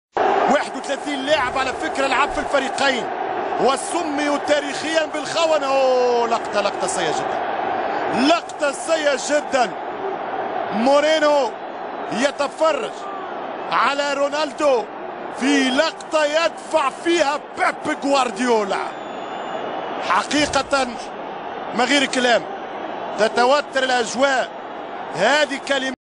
Cristiano Ronaldo pushes Pep Guardiola - Barcelona 5-0 Real Madrid - El Clasico 2010
Cristiano Ronaldo pushes Pep Guardiola - Barcelona 5-0 Real Madrid